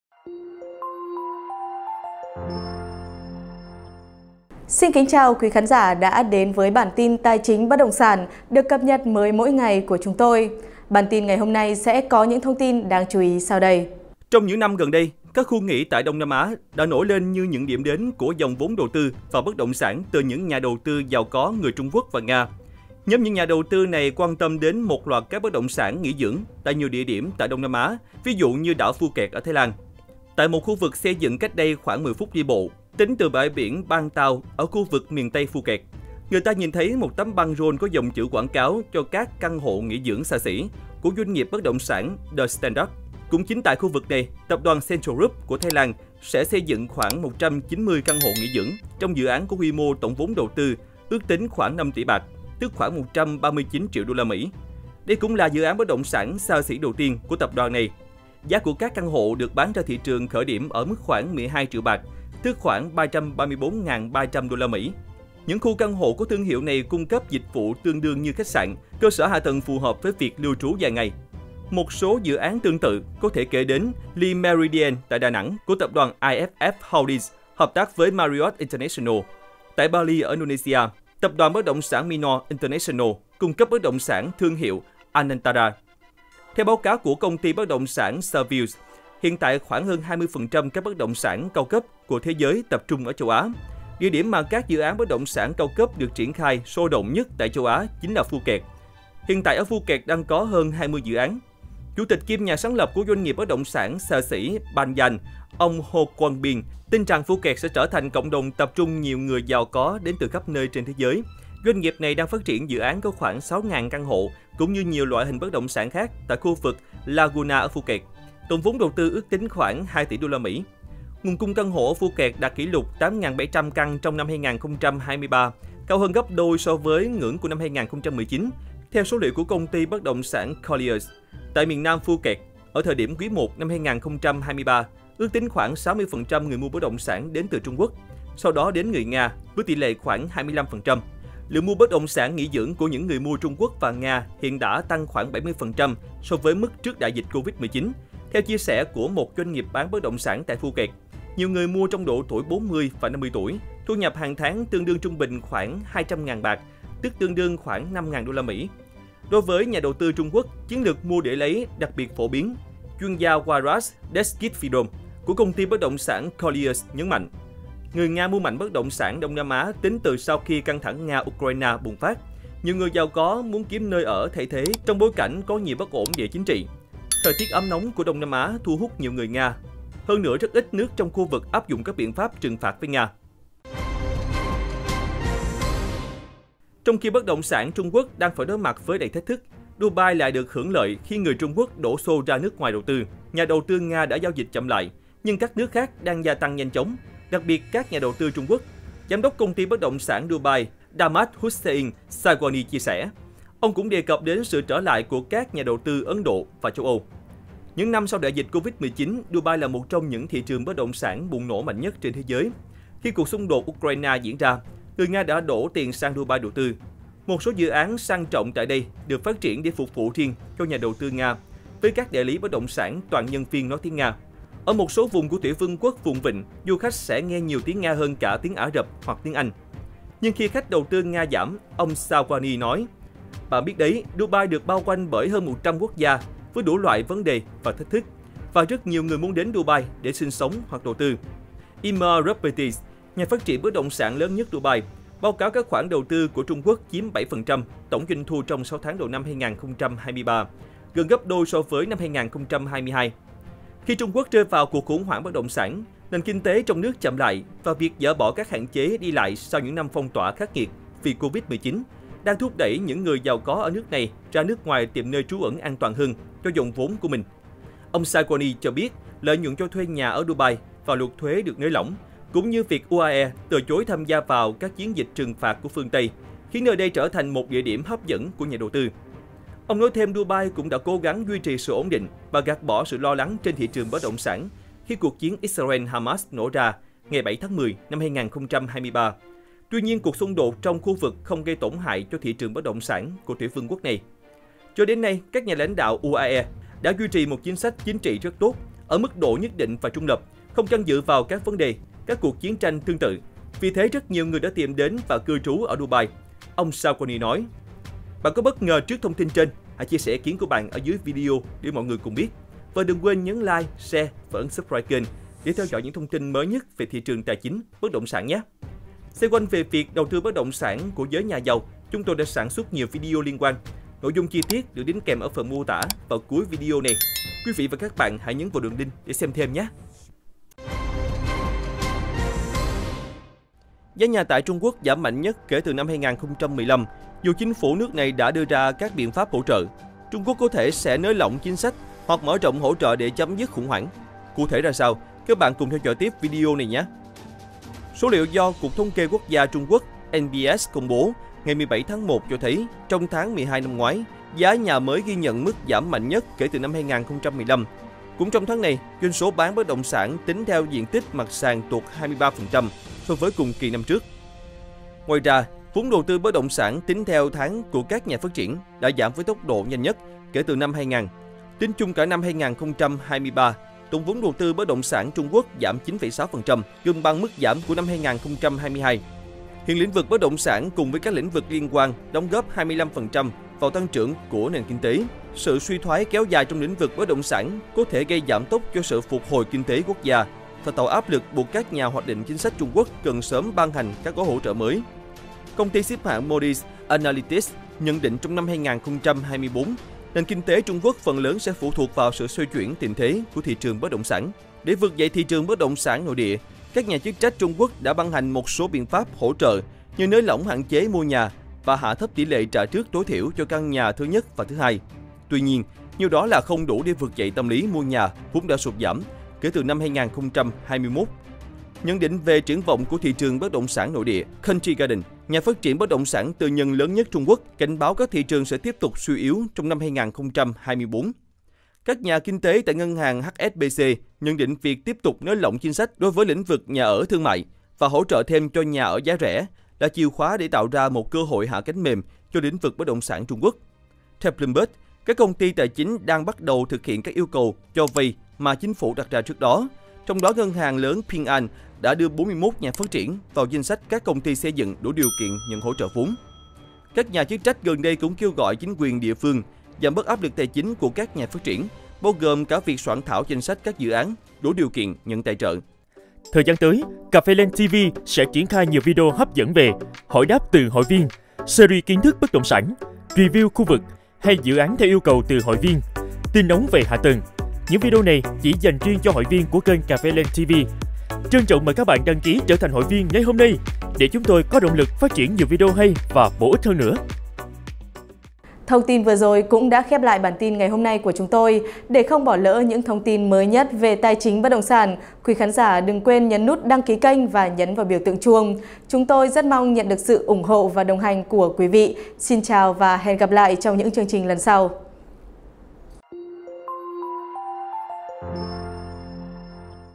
Vì sao GIỚI NHÀ GIÀU Trung Quốc và Nga mua mạnh các bất động sản tại Đông Nam Á? | CafeLand
Vì sao GIỚI NHÀ GIÀU Trung Quốc và Nga mua mạnh các bất động sản tại Đông Nam Á?

Trong những năm gần đây, các khu nghỉ tại Đông Nam Á đã nổi lên như những điểm đến của dòng vốn đầu tư vào bất động sản từ những nhà đầu tư giàu có người Trung Quốc và Nga. Nhóm những nhà đầu tư này quan tâm đến một loạt các bất động sản nghỉ dưỡng tại nhiều địa điểm tại Đông Nam Á, ví như đảo Phuket ở Thái Lan.